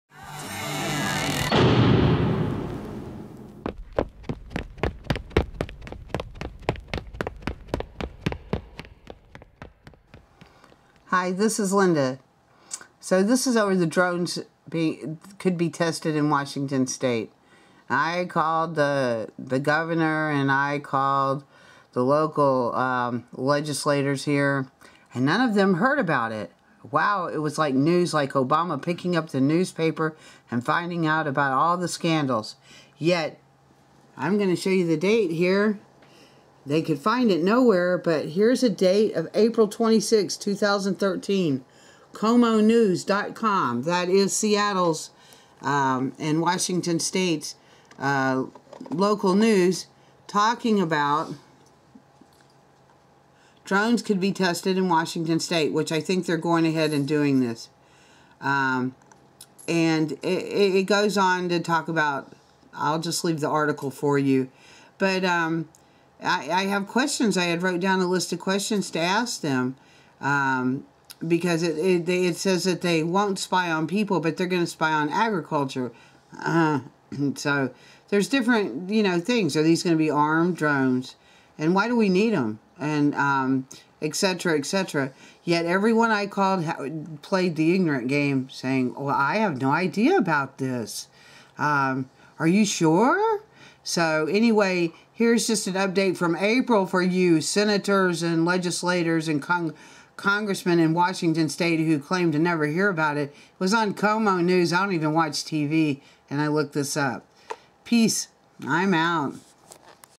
Update on Drones in wa st Legaslators never heard of this??? Obamas Syndrome
Every person i called seemed to never hear of the drones because they have not read the newspaper today ummmm, yet in April it was on the news...I left my phone number with each of these tax payer funded idiots
peace, love , truth
Link from KOMO news 4 in april on drones